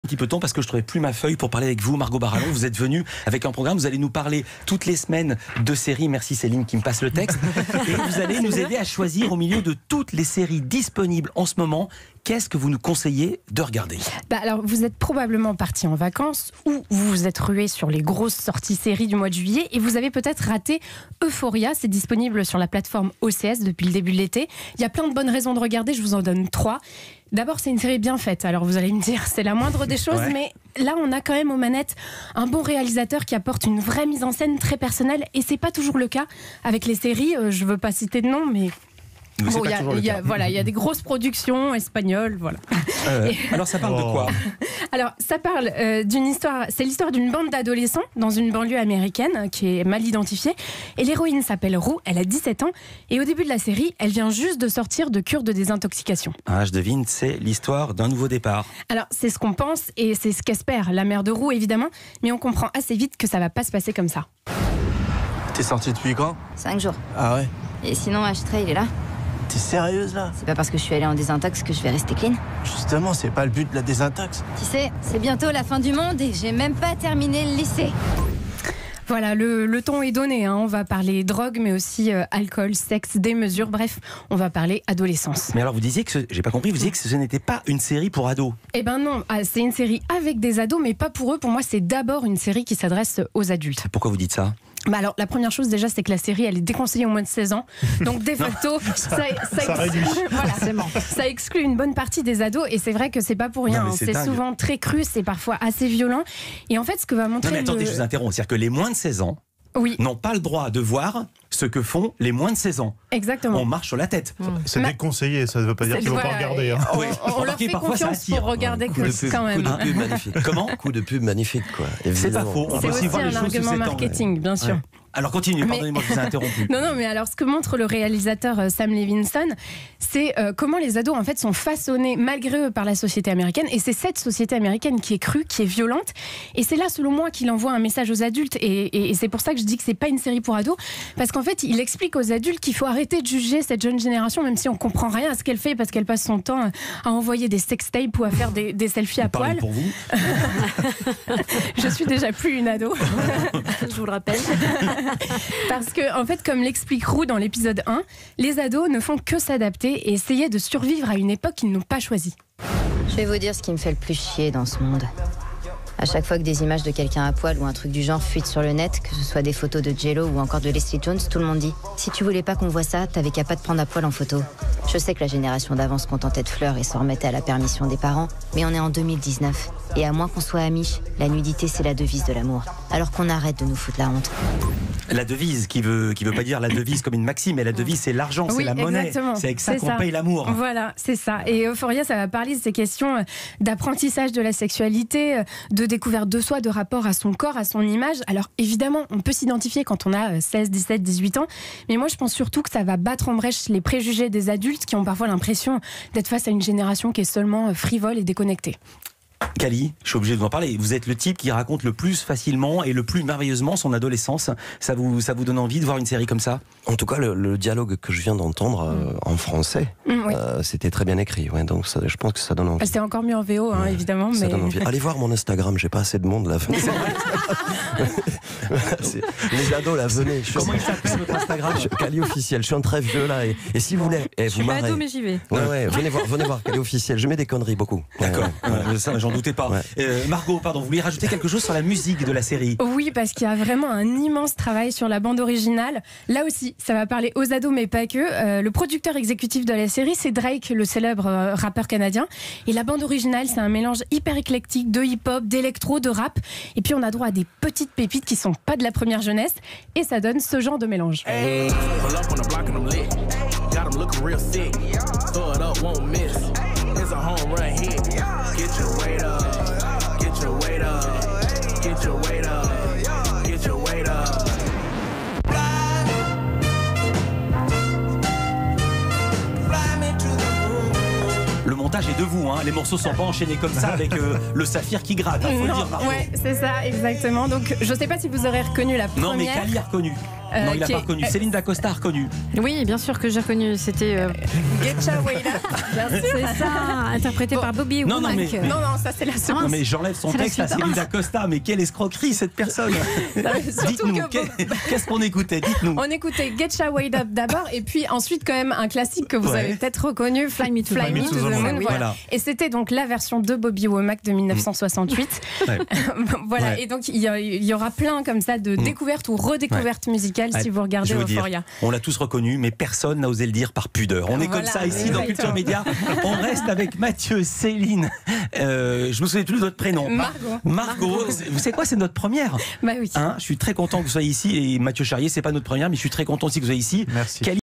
Euphoria, série d'ados pour adulte
La création de HBO met en scène la vie d'adolescents au lycée, confrontés aux problématiques d'apprentissage d'eux-mêmes et des autres. Paradoxalement, elle s'adresse plus aux adultes qu'aux grands enfants.